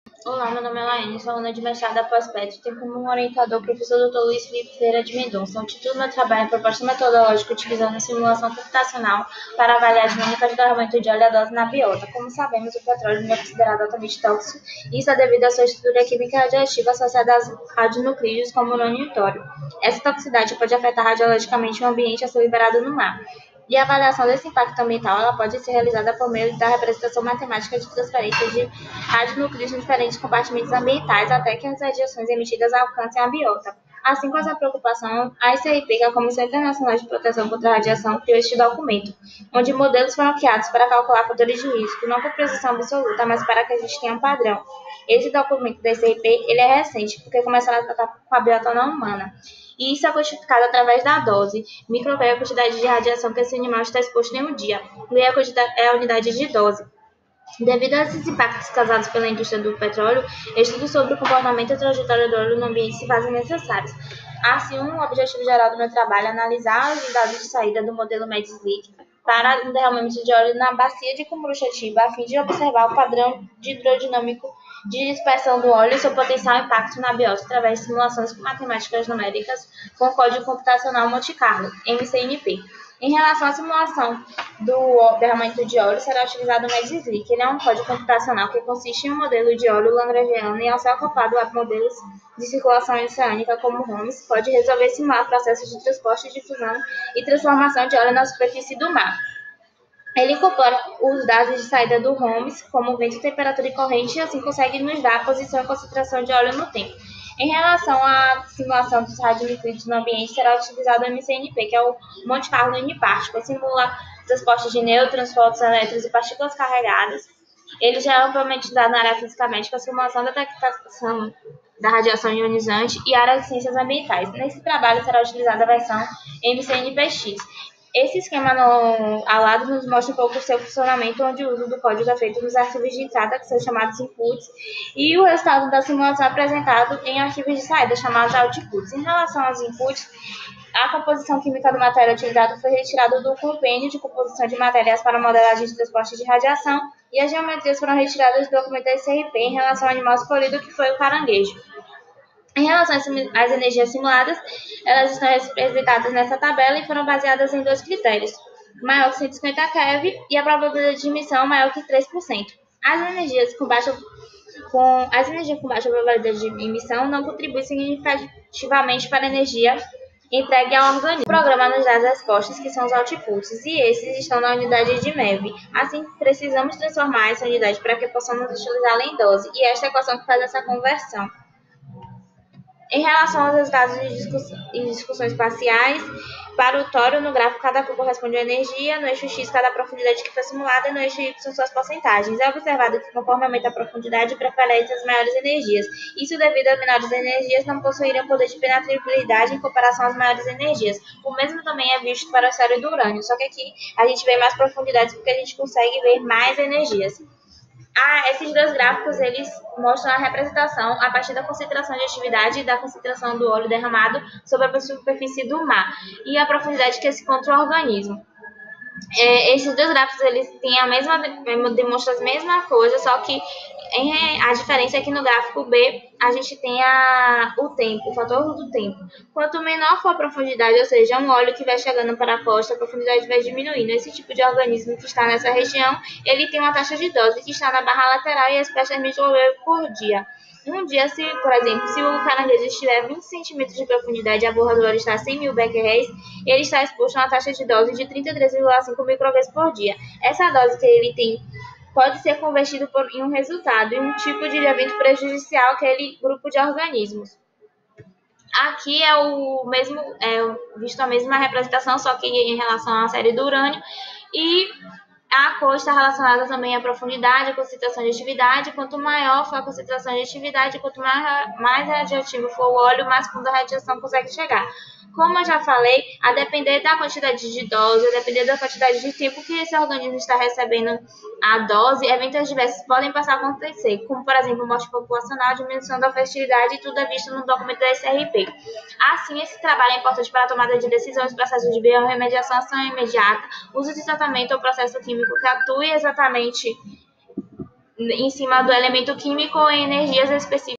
PROPOSTA METODOLÓGICA UTILIZANDO SIMULAÇÃO COMPUTACIONAL PARA AVALIAR DINÂMICA DE UM DERRAMAMENTO...
Título:
PROPOSTA METODOLÓGICA UTILIZANDO SIMULAÇÃO COMPUTACIONAL PARA AVALIAR DINÂMICA DE UM DERRAMAMENTO DE ÓLEO E A DOSE NA BIOTA

Autores:
LAIANNE DOS SANTOS PROTASIO, LUÍS FELIPE MENDONÇA

Descrição:
Os derramamentos de óleo correspondem a uma das mais impactantes fontes de poluição marinha. A liberação de óleo na superfície do oceano pode gerar potenciais impactos ambientais. Devido a estes impactos causados pela indústria de exploração de petróleo, conhecer seu comportamento no ambiente marinho através de estudos de prevenção, controle e mitigação de derramamentos de óleo se fazem necessários. Muitos estudos já fazem utilização de modelos e códigos computacionais que têm se mostrado uma importante ferramenta, de baixo custo computacional, para o gerenciamento de riscos. O presente projeto tem por objetivo analisar os dados de saída do modelo de dispersão de óleo no software MEDSLICK-II, através do estudo de caso de um derramamento de óleo na região da Bacia de Cumuruxatiba. Este modelo calcula a partir de dados de um modelo regional oceânico o transporte e intemperismo do óleo, utilizando uma representação Lagrangiana da mancha. Além disto, propomos observar o impacto do óleo na biota através do código computacional Monte Carlo (MCNP), que estima o transporte de radiação ionizante do óleo para biota no meio oceânico. O estudo será desenvolvido para a Bacia de Cumuruxatiba, região ao sul do estado da Bahia, escolhida devido a sua proximidade com o Parque Nacional do Abrolhos e o alto risco ambiental associado a indústria do petróleo na região. Com base nestas informações geradas pelos modelos se almeja criar uma metodologia de aplicação para futuros estudos de derramamentos de óleo e seu impacto nas áreas costeiras do litoral brasileiro.